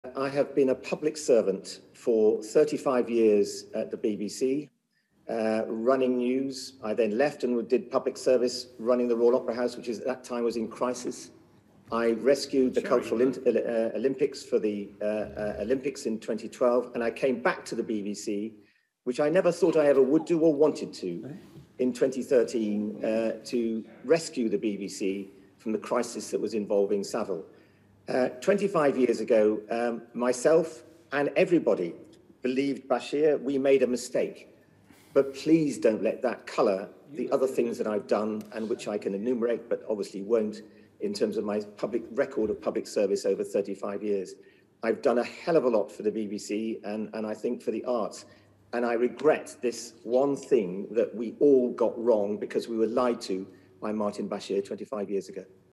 BBC Diana Hearing: Lord Hall defends his credibility
Former BBC Director-General Lord Tony Hall has told MPs that he "trusted" Martin Bashir and "gave him a second chance" - but that trust was "a mistake".

Lord Tony Hall spoke to the Digital, Culture, Media and Sport Committee about events leading up to Mr Bashir's now infamous Panorama interview with Diana, Princess of Wales, in 1995.

But he said that he hoped that his mistake wouldn't detract from his years of public service.

and download our apps